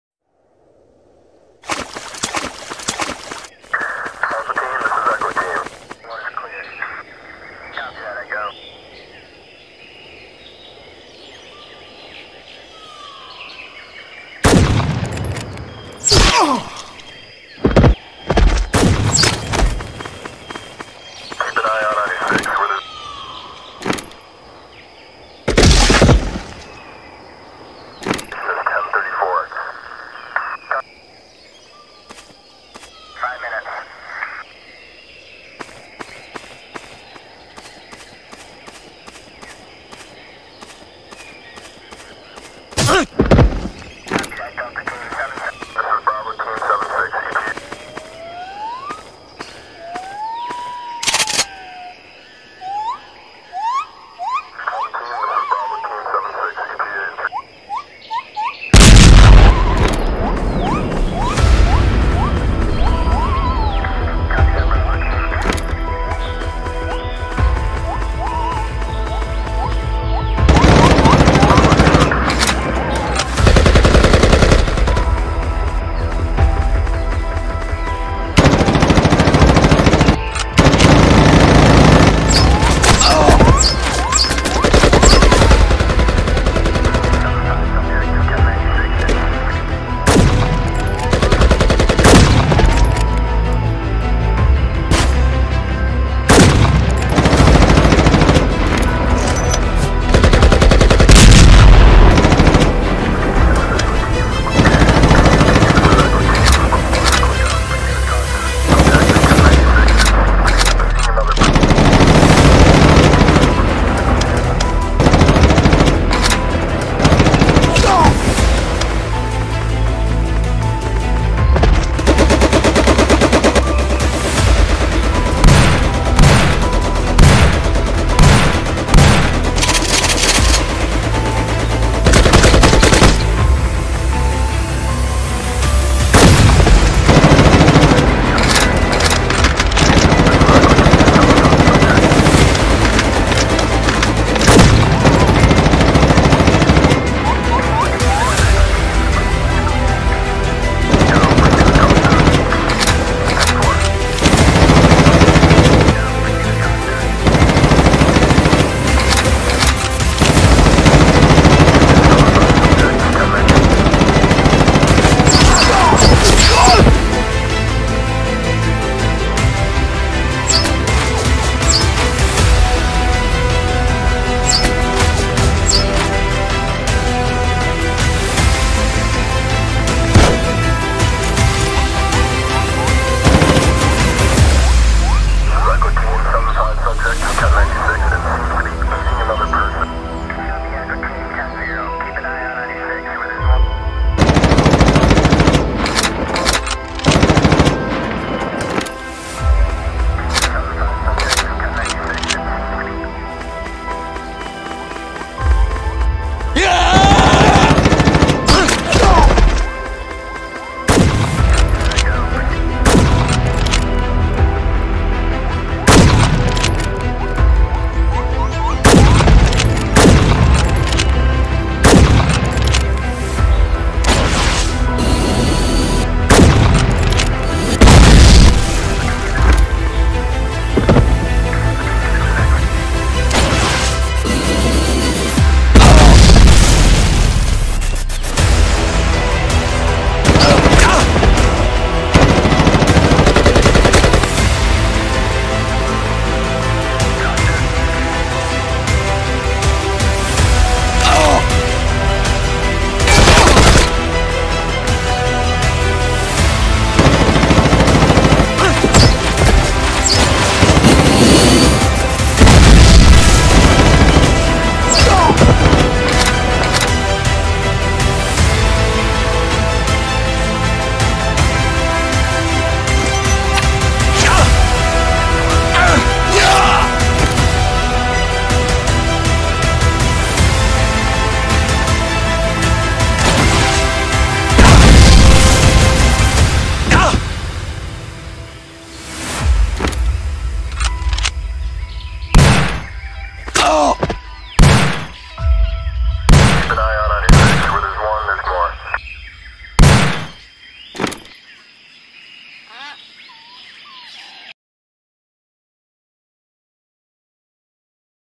StickNodes War ELITE FORCE vs REBELS | Stick nodes war
Hi guys, im back.. sorry for not upload last week. im too busy at my work. so, this day i will upload a war video, something special for my Indonesian 🇮🇩 subscriber and viewers, also you guys. The Kopassus, it is a legendary Elit force from Indonesia. so, i make it in animation style. And, you have any request just put on comment section.